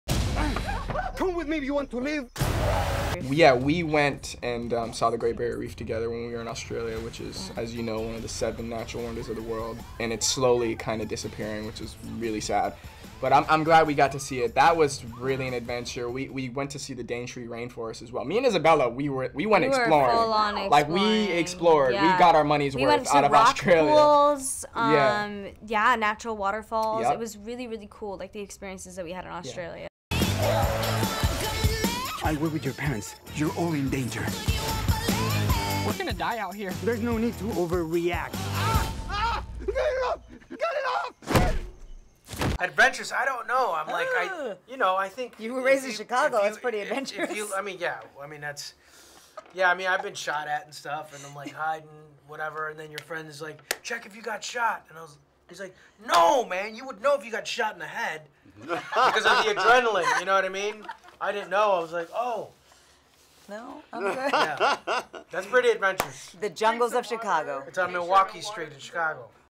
'Dora' stars' big adventures
"Dora" stars Isabela Moner and Jeff Wahlberg visited the Great Barrier Reef, but Michael Pena's biggest real-life adventures have been more urban. (Aug. 9)